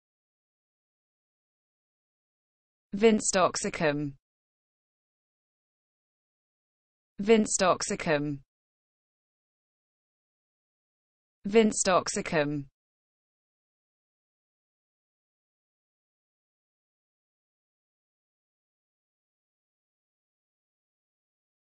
vincetoxicum: Pronounce vincetoxicum with Meaning, Phonetic, Synonyms and Sentence Examples
This video shows pronunciation of vincetoxicum in a sentence, vincetoxicum meaning, vincetoxicum definition, vincetoxicum phonetic, vincetoxicum synonym and vincetoxicum example

vincetoxicum Definition/Meaning : genus of chiefly tropical American vines having cordate leaves and large purple or greenish cymose flowers; supposedly having powers as an antidote